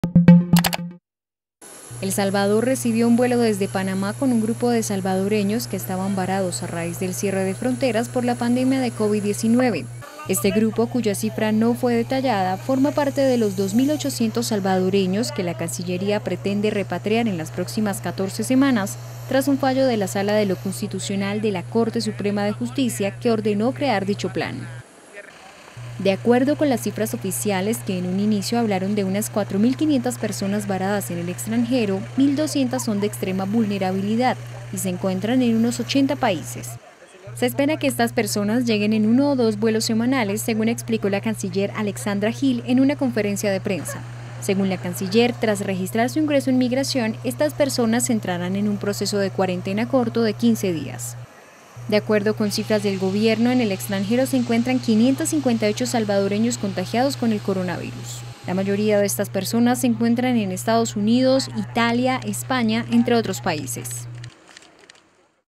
Retorna a El Salvador un grupo de personas varadas en exterior por coronavirus
El Salvador recibió este lunes un vuelo desde Panamá con un grupo de salvadoreños que estaban varados a raíz del cierre de fronteras por la pandemia de COVID-19, informó la Cancillería del país centroamericano.

¡Visítenos!